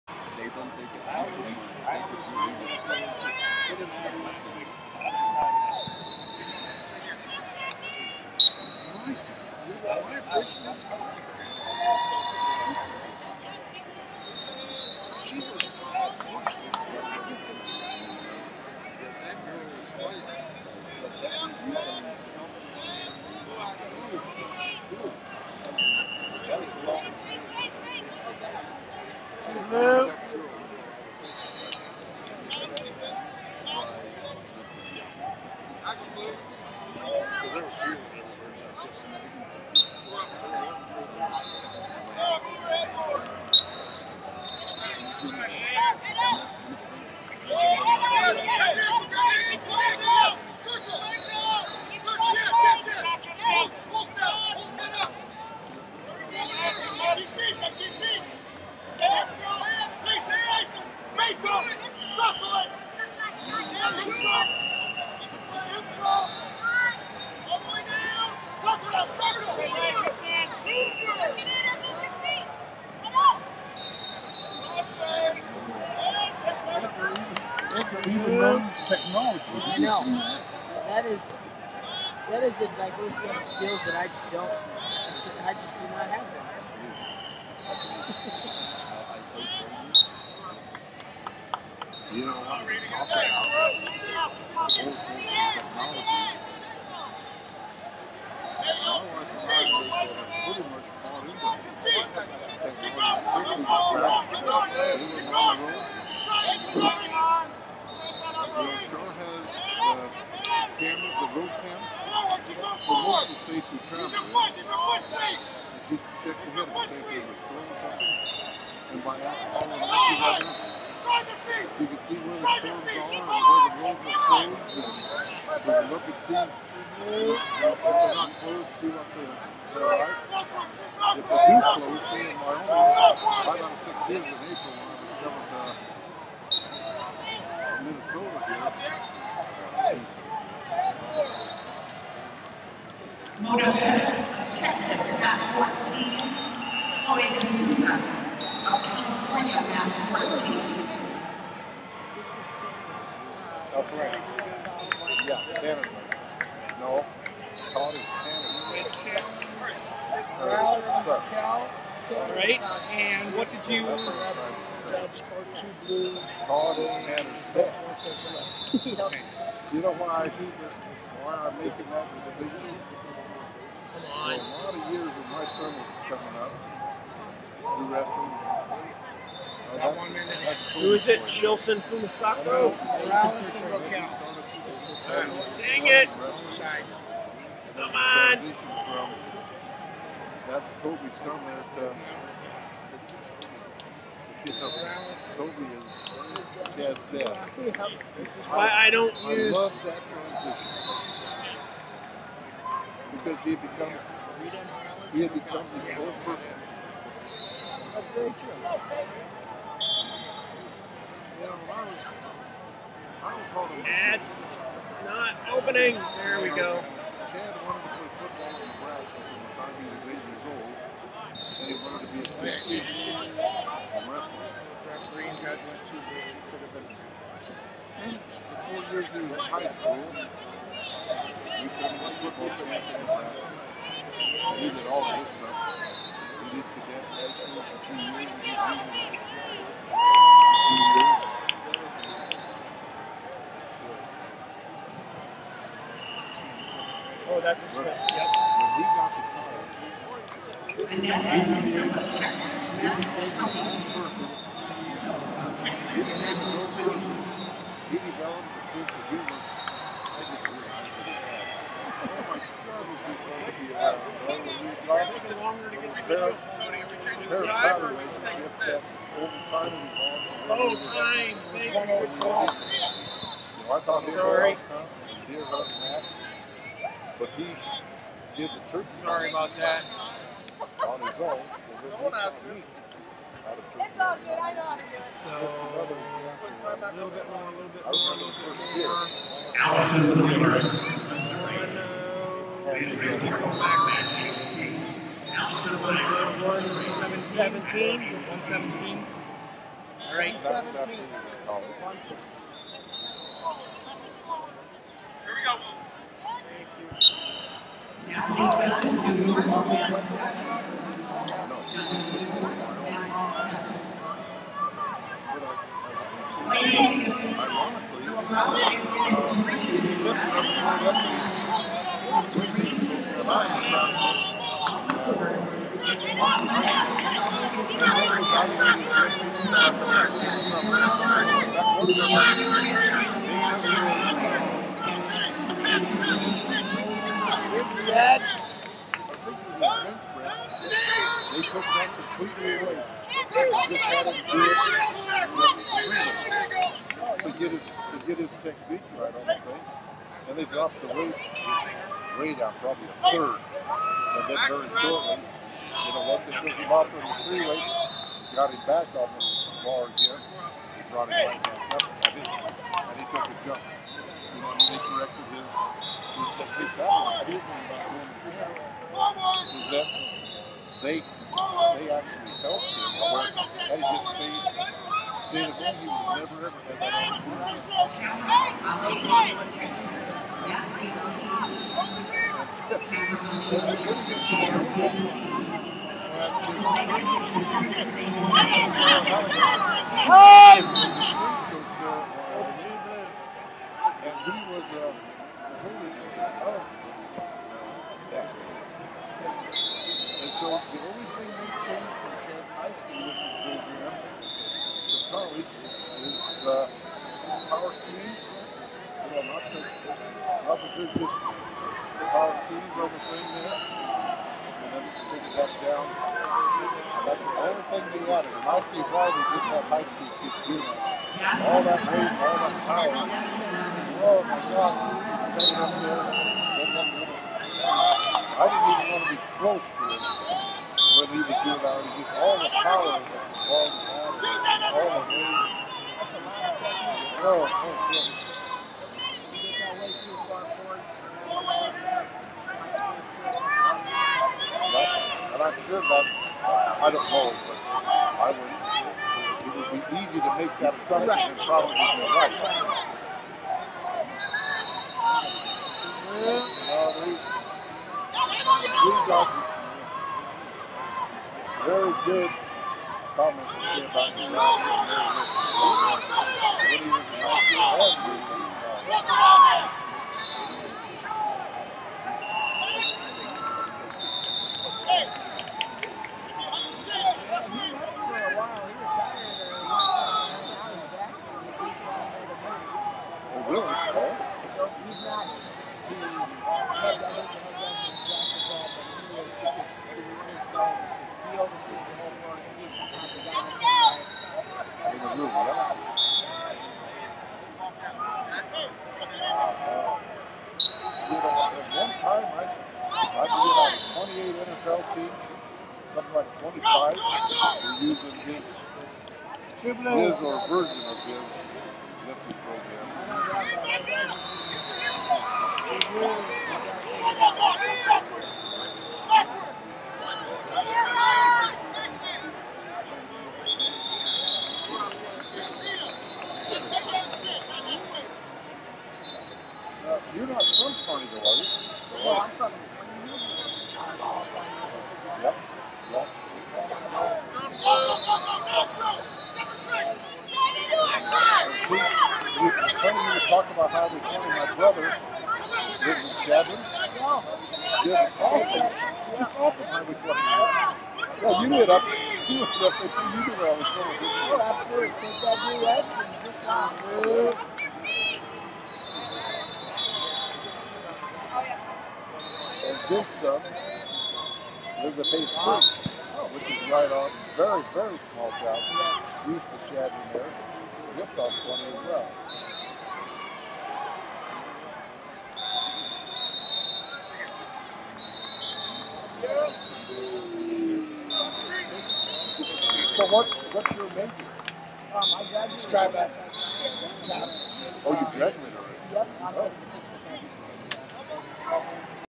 117 Lbs Rnd Of 64 Rita Morales Hawaii Vs Ciera Broukal Indiana
2019 USMC USAW Junior And Cadet National Championships - 117 Lbs Rnd Of 64 Rita Morales Hawaii Vs Ciera Broukal Indiana